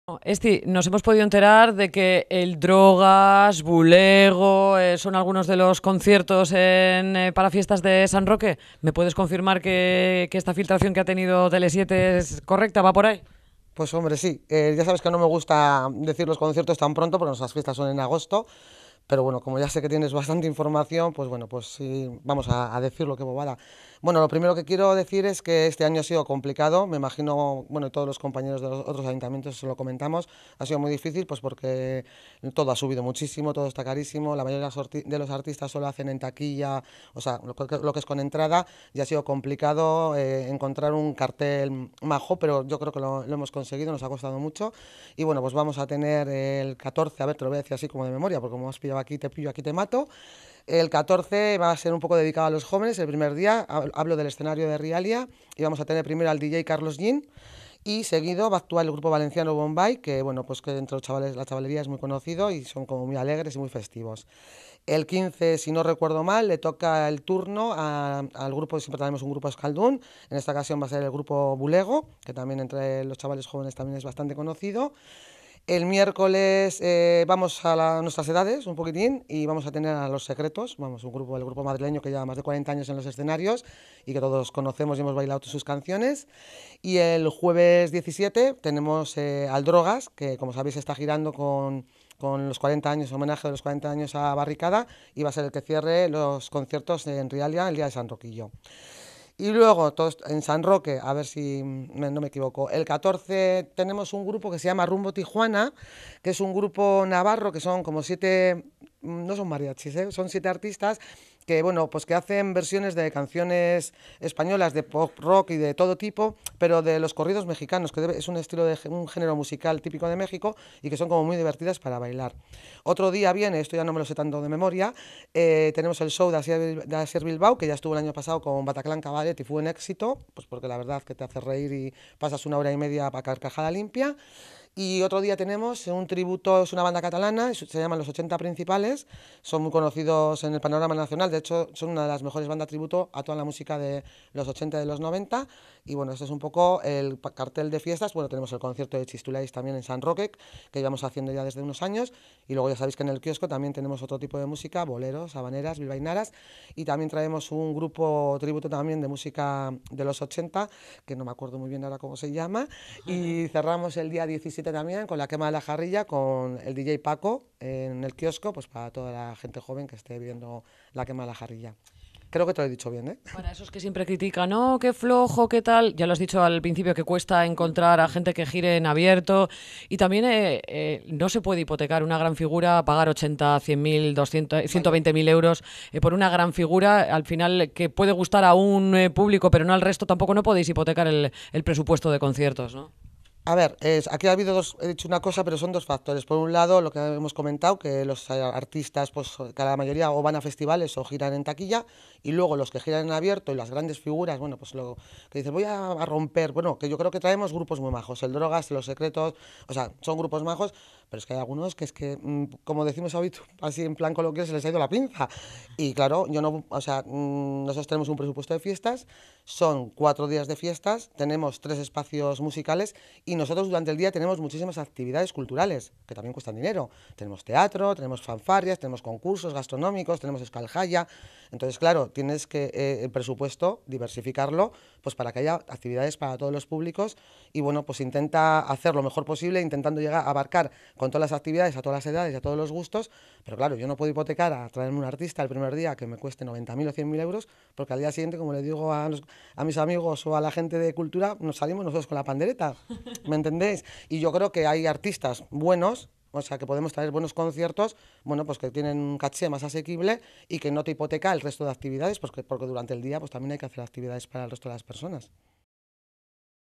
Los Secretos, El Drogas, Carlos Jean, Bulego y Bombai; Conciertos de fiestas de Portugalete
Se ha optado por un programa variado, que amplíe el abanico a más gustos musicales.